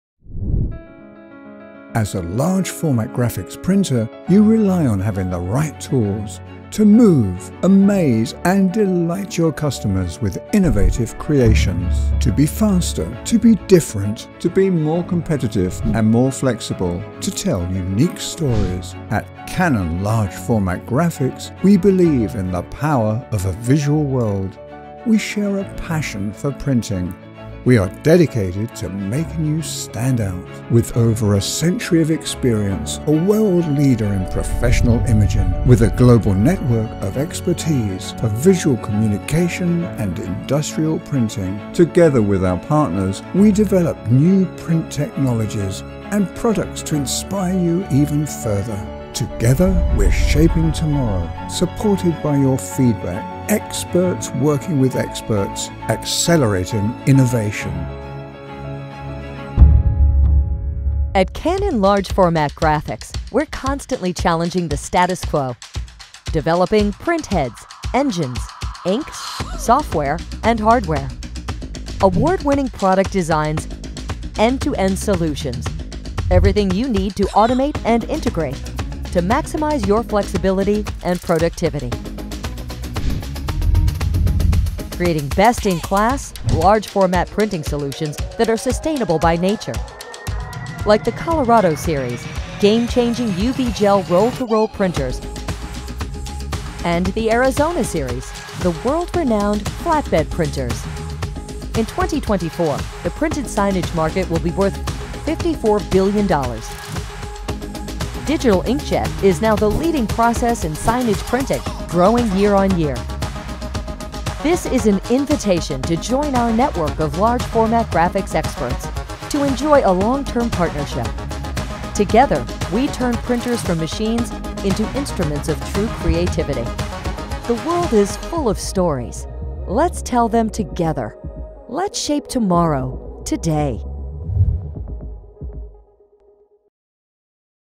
Exciting growth opportunities for large format graphics
As a Large Format Graphics printer, you rely on having the right tools to move, amaze and delight your customers with innovative creations. Tools that help you innovate and catch the eye, so you can move ahead of the competition while earning a healthy profit. The right tools enable you to create captivating stories that bring your customers back again and again.

At Canon Large Format Graphics we believe in the power of a visual world. We share a passion for printing and are dedicated to making you stand out. Now, in 2020, our Arizona series and Colorado series continue to inspire creativity and bring out the best in our customers. In this video, you can take a look at how our unique approach is driving innovation in the impressive Large Format Graphics growth market and how Canon helps businesses take advantage of the huge opportunities this market has to offer.